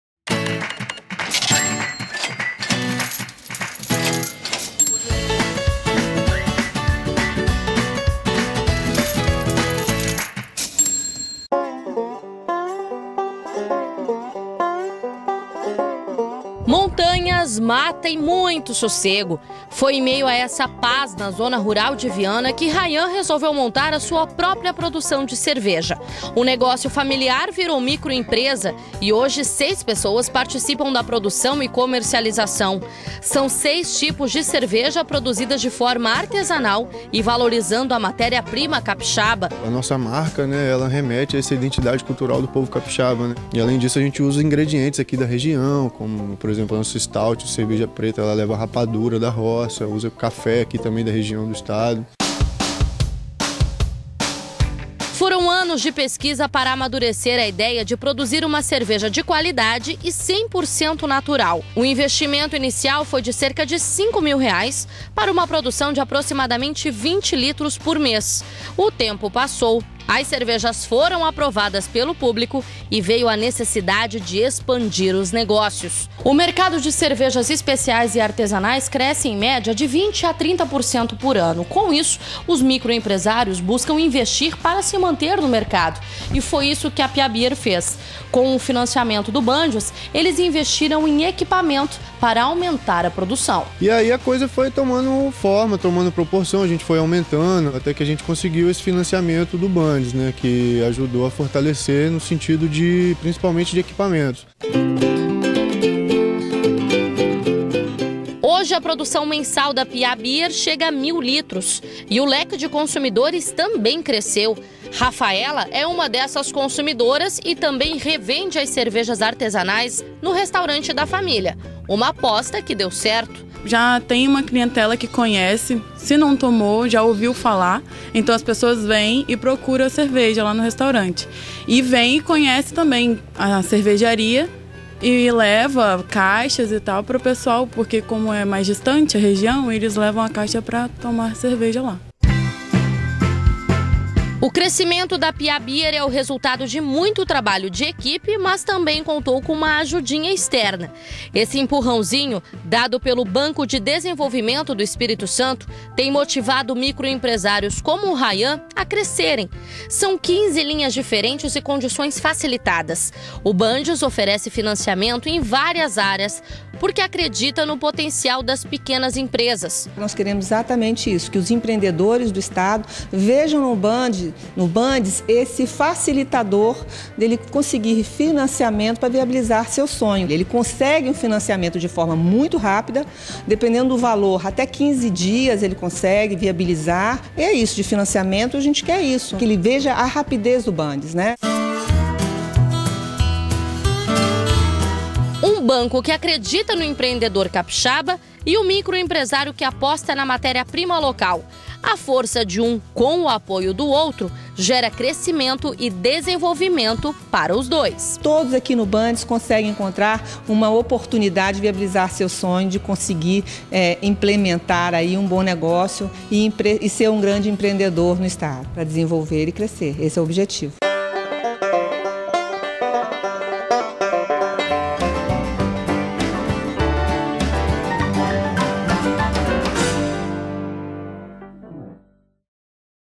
Economia criativa
Uma cerveja artesanal, 100% capixaba. O investimento inicial, de cinco mil reais. Ficou assustado? Então, acompanhe essa história ela pode te inspirar.